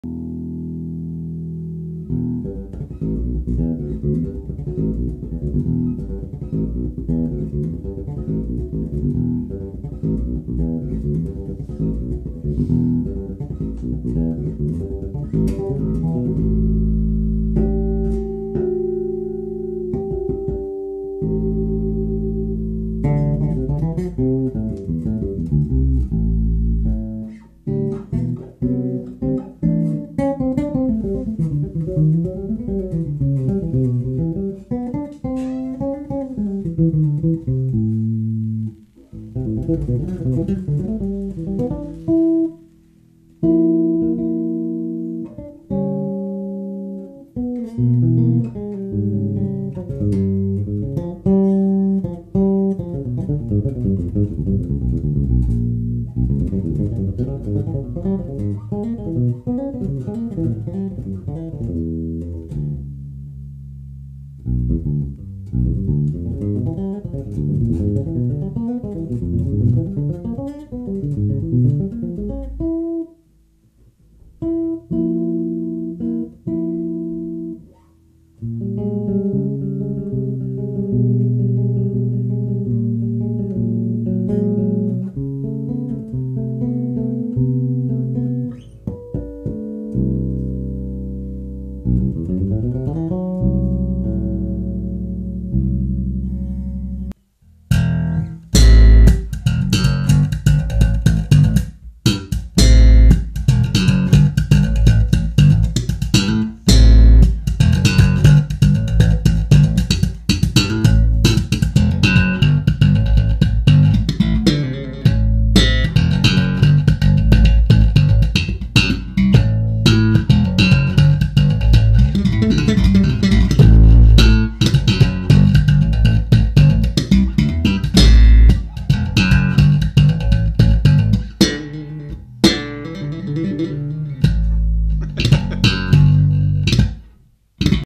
New BeBop for Glenn Gaddum jr
Glenn came in to pick up his custom made BeBop recently. When he was checking it out we quickly realised we needed to get our camera! This BeBop has an alder body, maple neck, bindings + blockinlays, NJ4 pickups and our standard hardware and Fatboost. Glenn requested a stringspacing of 18 mm, so that's whhat we did. His is the only BeBop that has that. Standard is 19.5 mm.